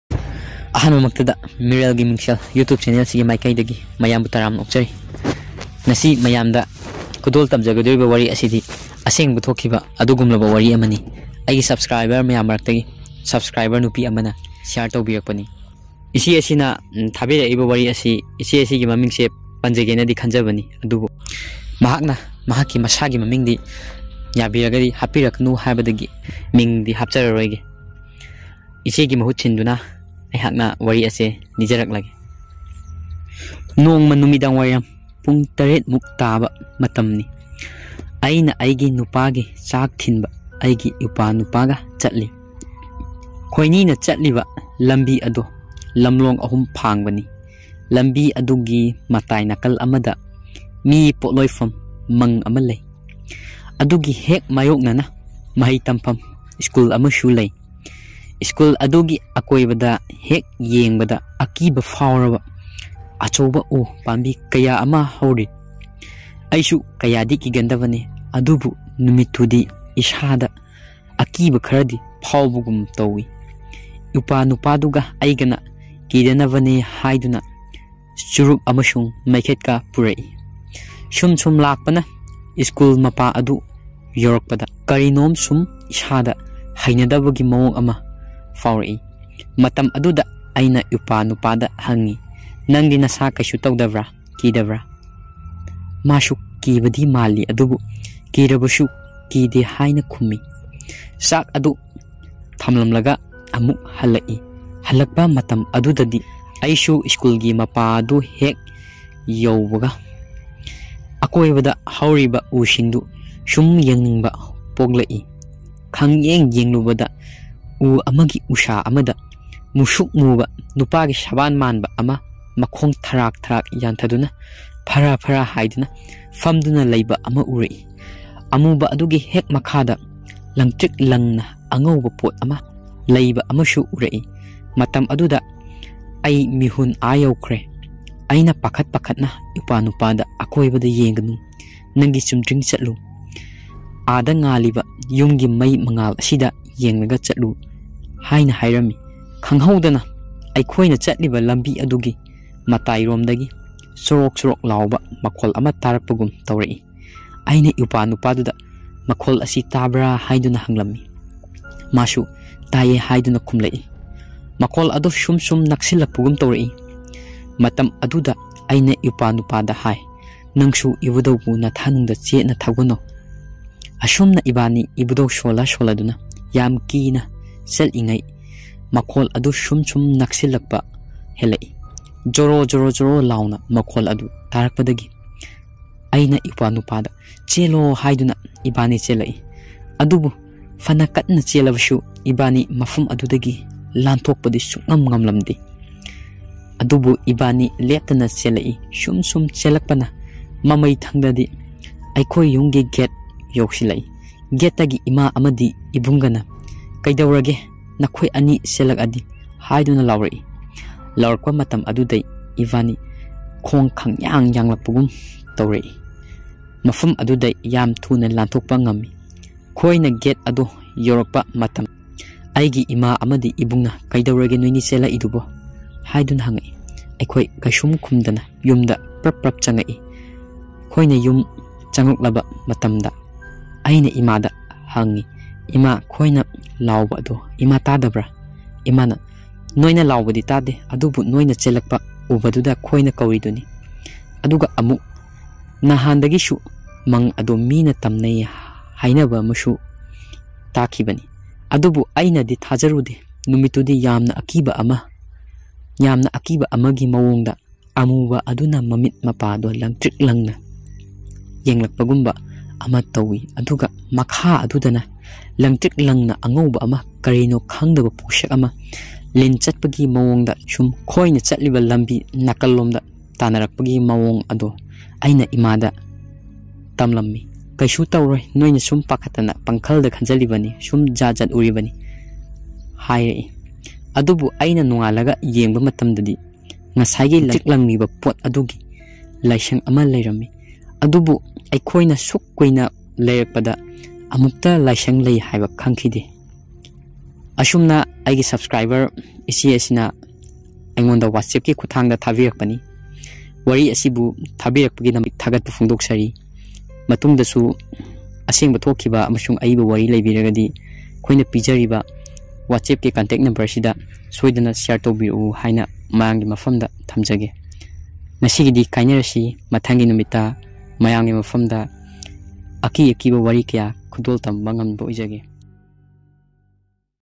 "Chak Thillu Hallakpada" || A Manipuri True Horror Story, Episode-1 || Please Subscribe and Share
Narrator : Ningthoujam Bikash Singh
Like and Follow on official fb page Meerel Gi Mingsel